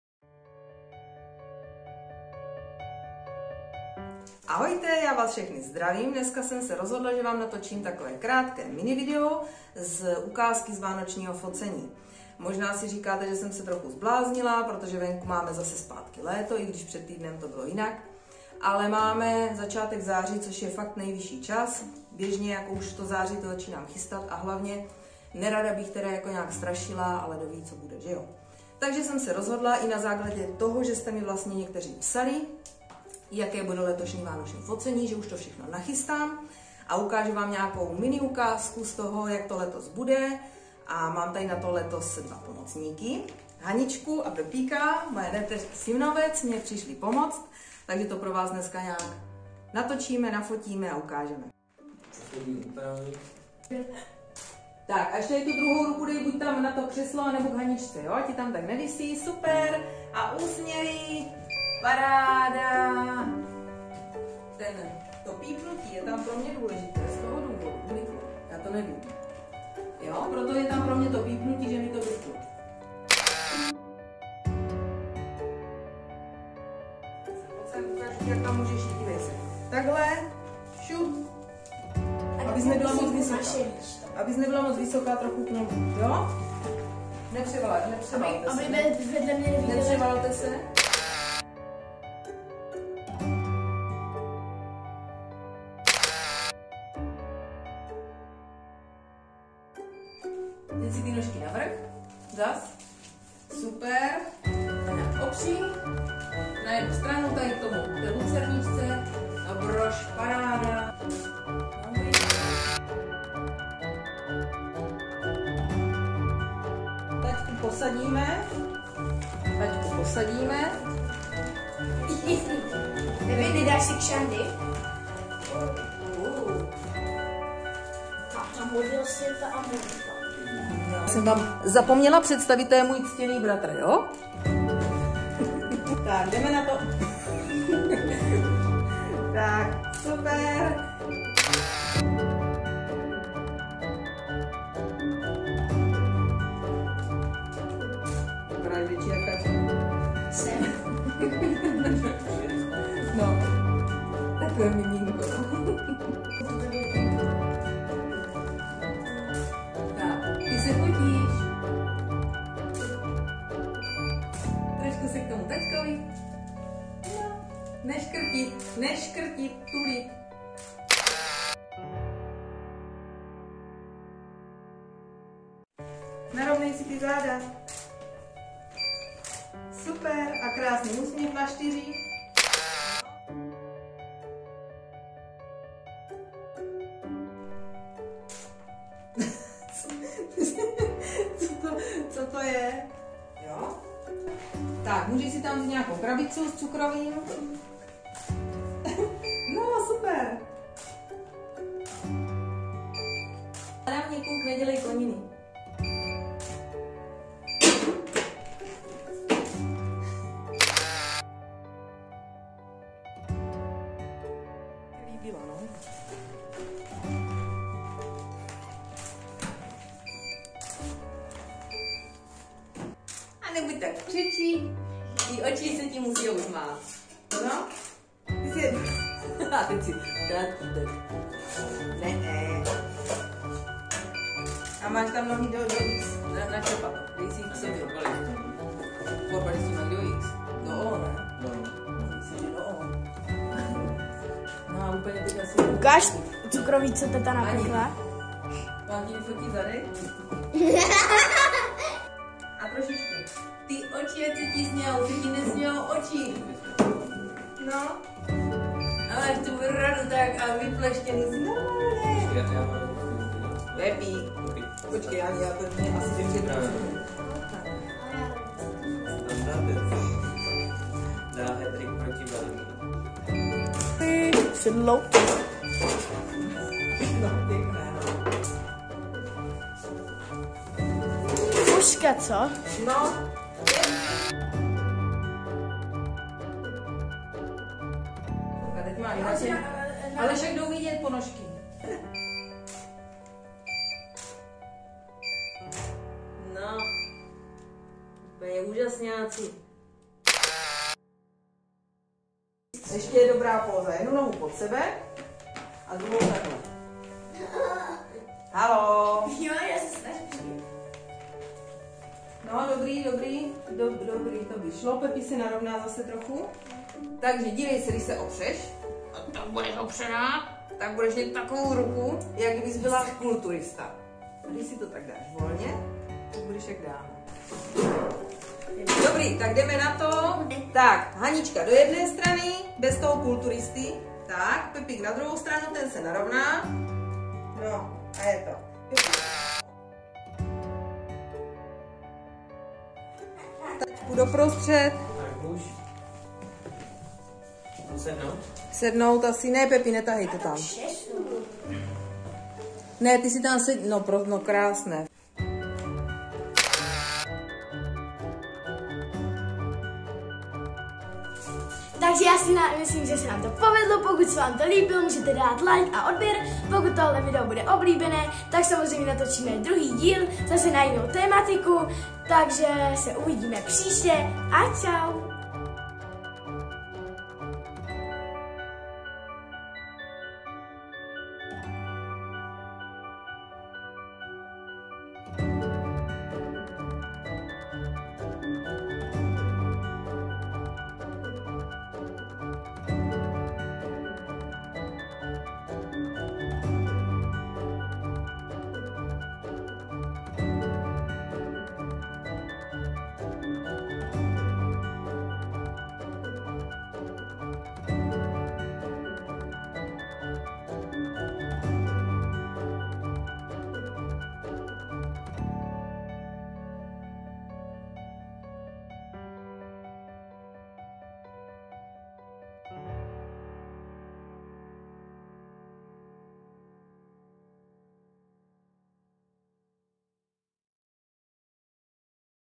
Vánoční focení 2021 - ukázka jak to probíhá při focení
Pro letošní Vánoce 2021 jsem vám nachystala zase dvě úplně nové fotopozadí, nebo chcete-li aranže :) Každý si může vybrat, jestli chce raději světlé a víc zdobené, nebo spíš tmavší dřevěné spíš víc pohádkové. Fotím u mě v ateliéru, nebo jezdím fotit na jiné místa, kam si mě pozvete. Třeba mateřská školka nebo dětský klubík atd...  Moc doufám, že se vám letošní focení bude zase líbit a zůstanete mi věrní :)